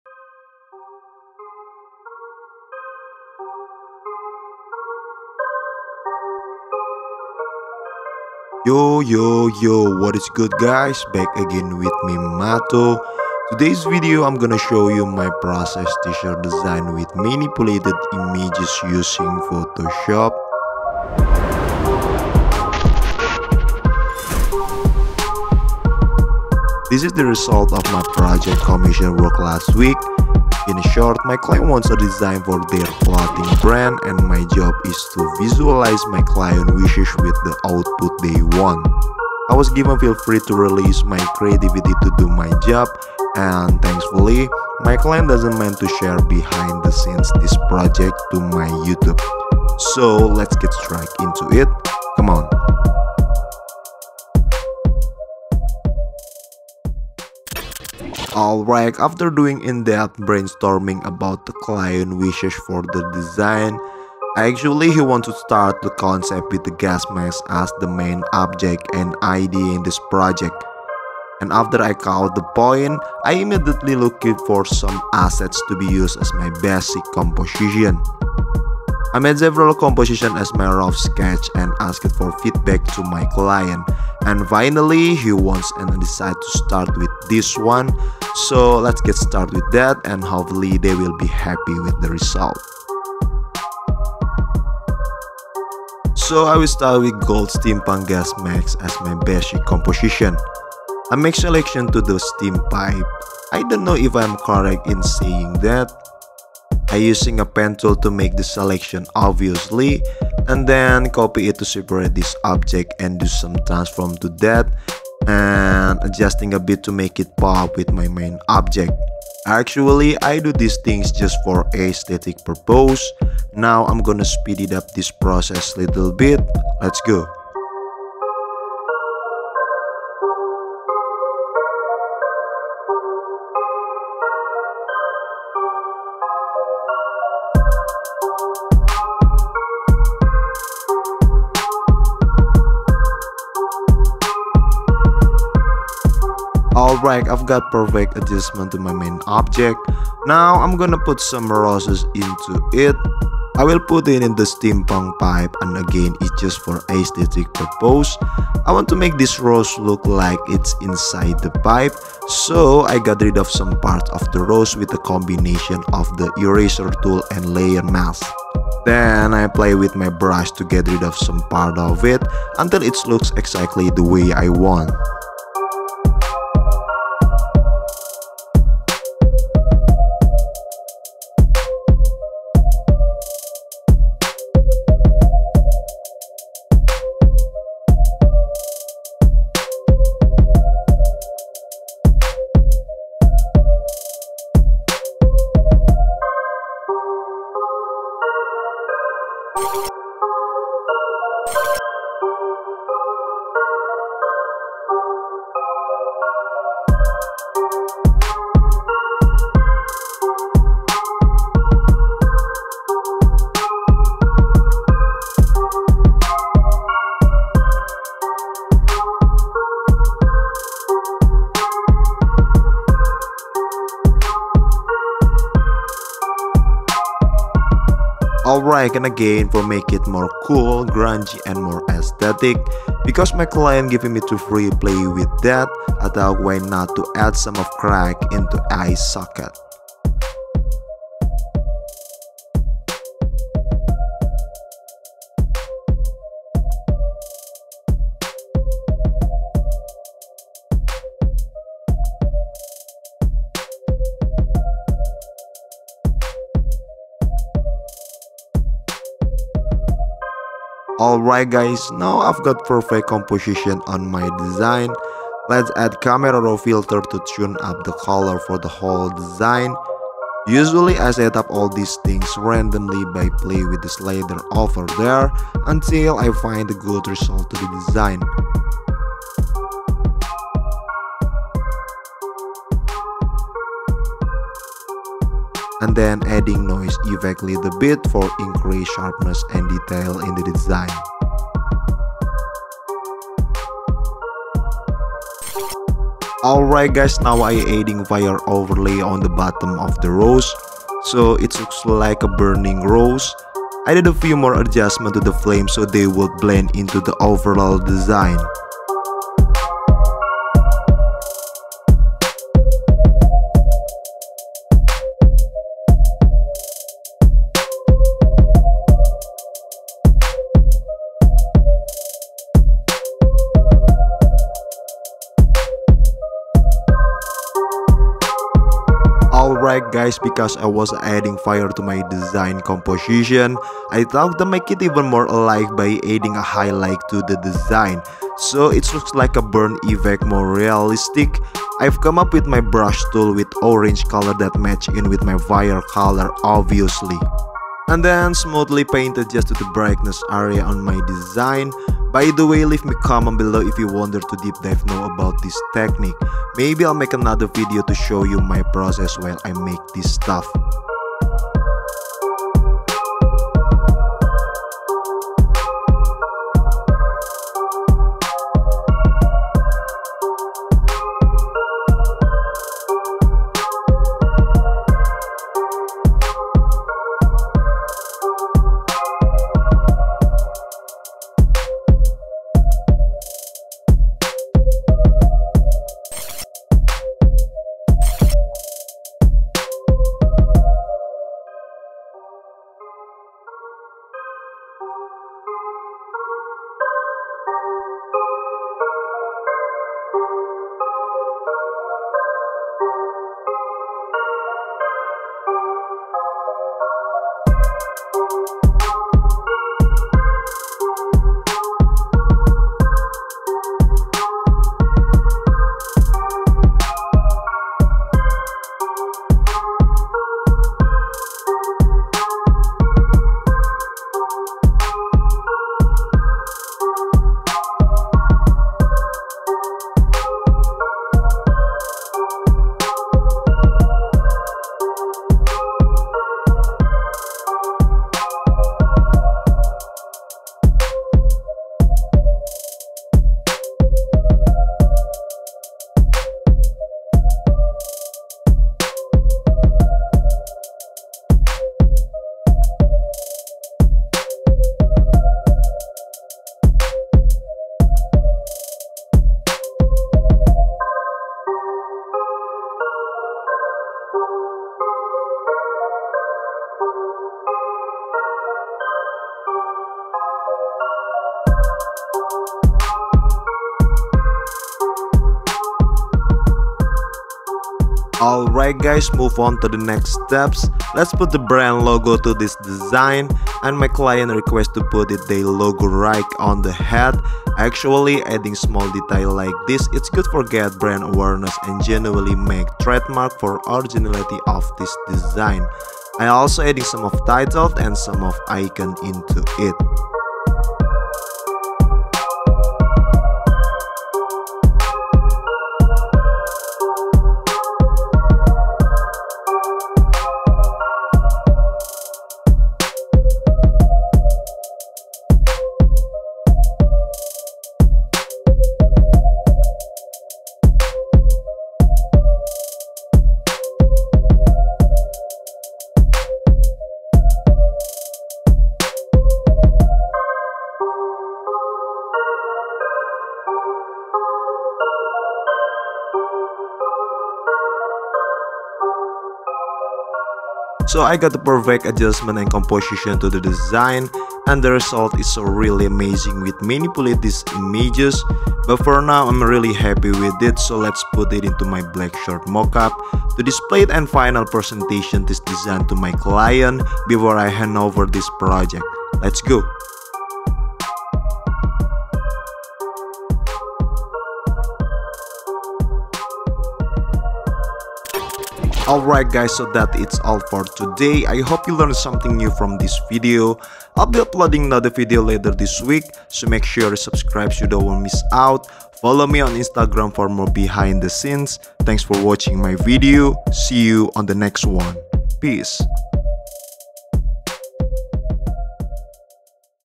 Photoshop Manipulation For Clothing Brand | Process T-Shirt Design
In this video, I will designing an original Merch Design into realistic way using Adobe Photoshop! Combine several elements into one, resulting a unique designs for a tshirt.

its makes a difference. i really appreciate for your support guys.